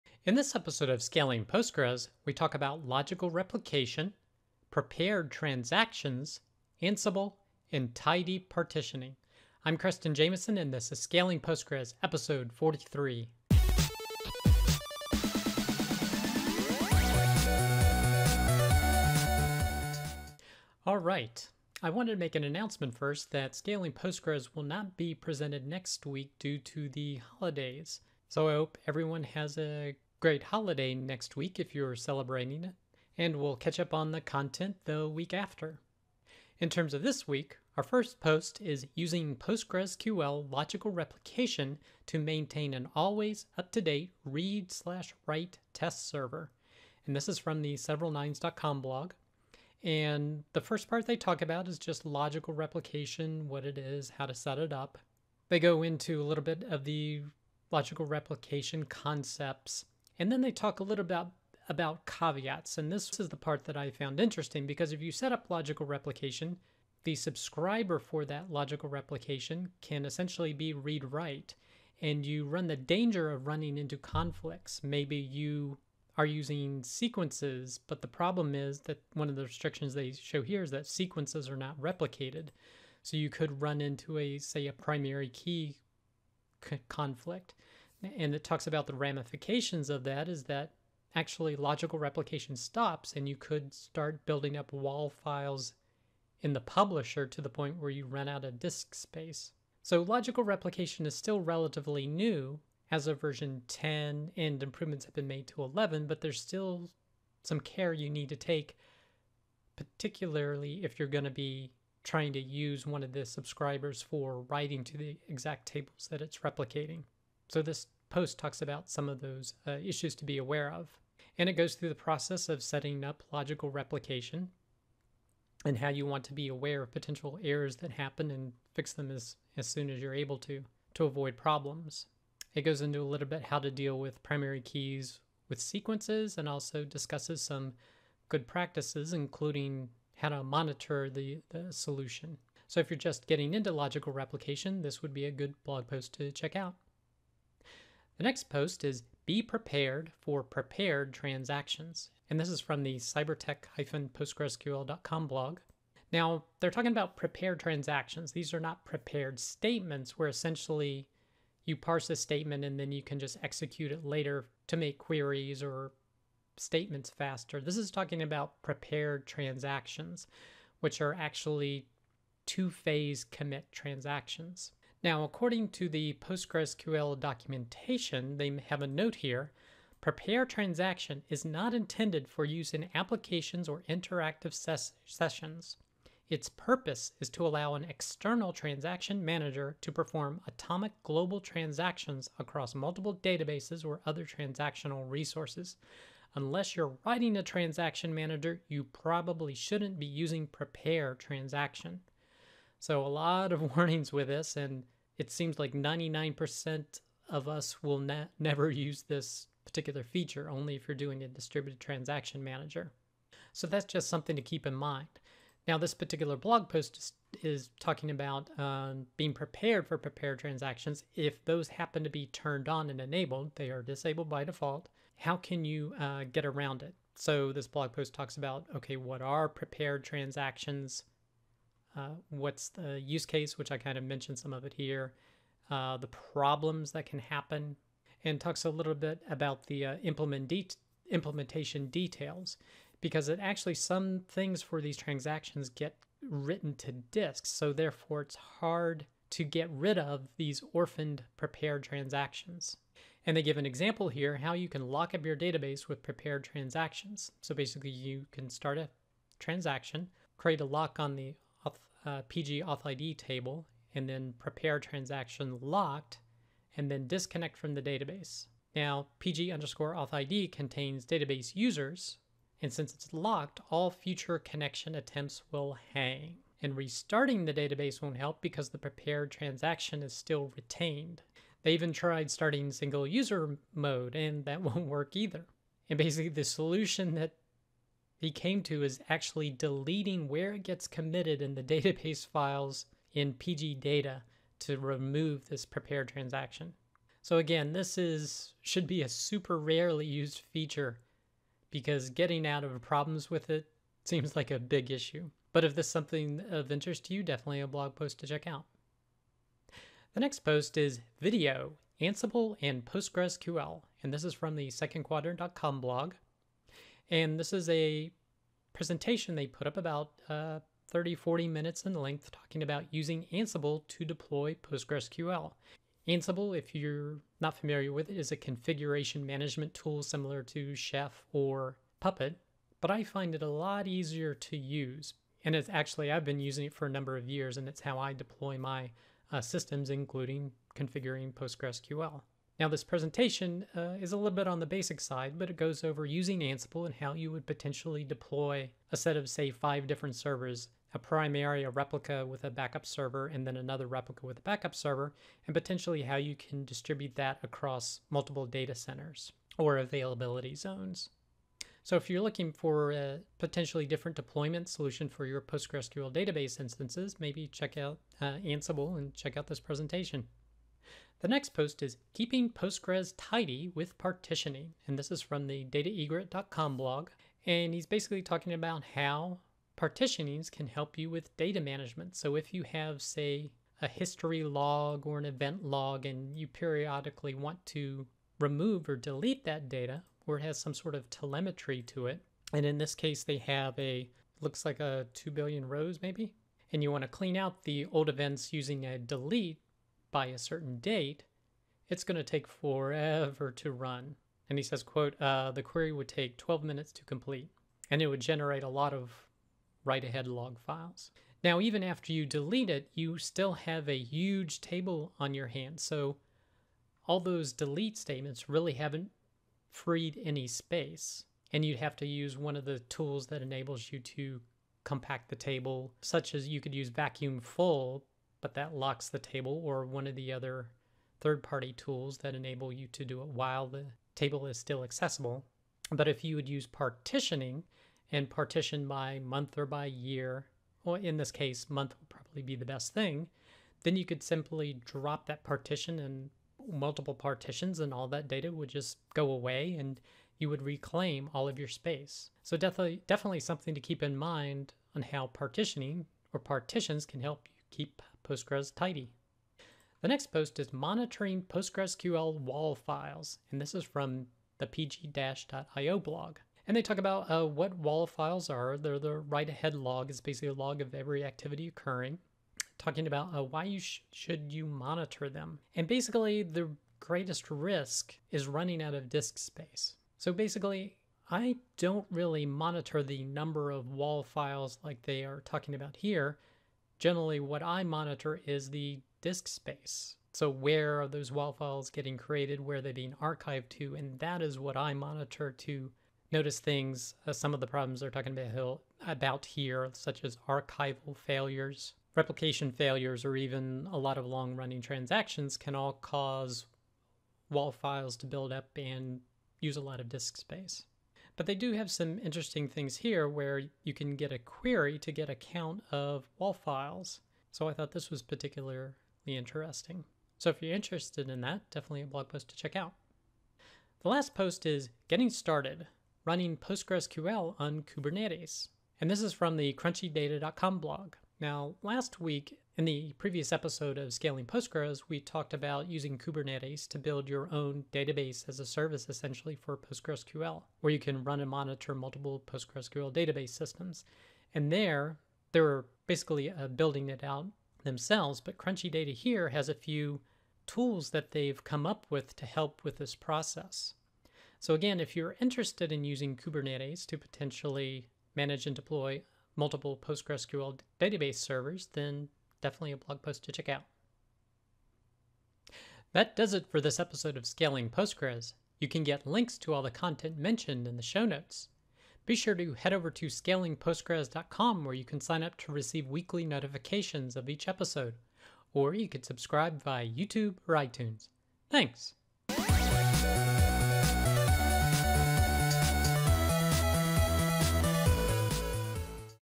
Scaling Postgres Episode 43 | Logical Replication | Prepared Transactions | Ansible | Partitions
In this episode of Scaling Postgres, we review articles covering logical replication, prepared transactions, Ansible Postgres deployment Ansible and tidy partitions.

Want to learn more about Postgres performance?
Join my FREE mini-course called the PostgreSQL Performance Starter Kit

Timestamps:
00:00 - Intro
00:37 - Using PostgreSQL Logical Replication to Maintain an Always Up-to-Date Read/Write TEST Server
02:27 - Be prepared for prepared transactions
05:09 - [Video] Ansible and PostgreSQL
06:11 - Keeping Postgres tidy with partitioning
07:40 - Monitoring PostgreSQL WAL Files
08:43 - Get Started Running PostgreSQL on Kubernetes
09:25 - Outro